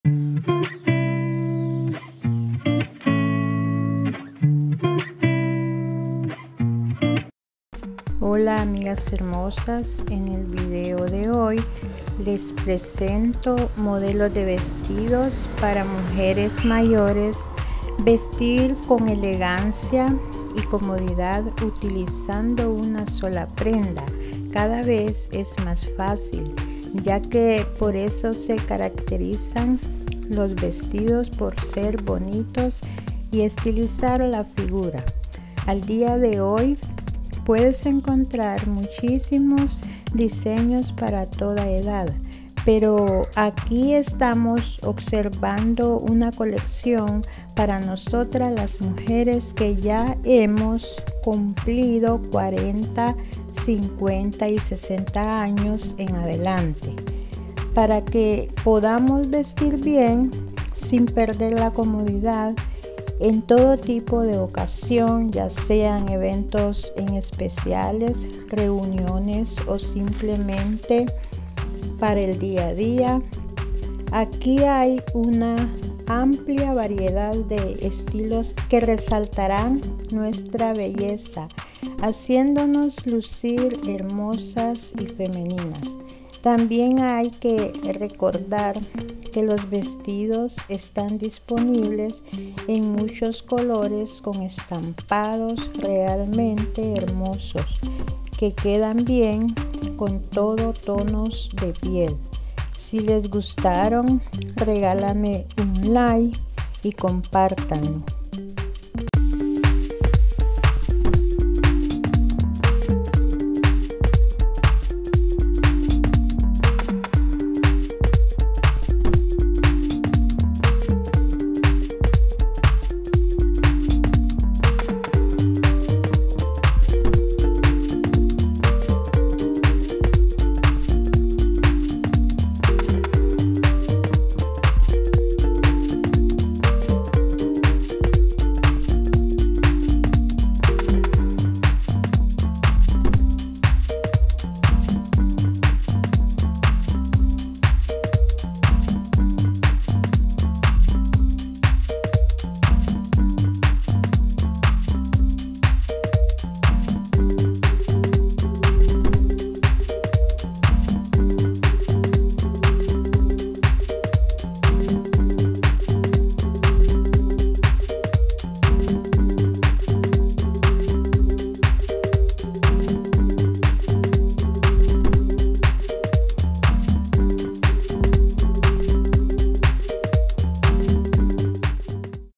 MODELOS de VESTIDOS LINDOS para DAMAS de 45+50-65 sencillos
hola amigas hermosas en el video de hoy Les presento modelos de vestidos para mujeres mayores vestir con elegancia y comodidad utilizando una sola prenda cada vez es más fácil ya que por eso se caracterizan los vestidos por ser bonitos y estilizar la figura al día de hoy puedes encontrar muchísimos diseños para toda edad por aquí estamos observando una colección para nosotras las mujeres que ya hemos cumplido 40 50 y 60 años en adelante para que podamos vestir sin perder la comodidad en todo tipo de ocasiones ya sea en eventos especiales reuniones o simplemente para el día a día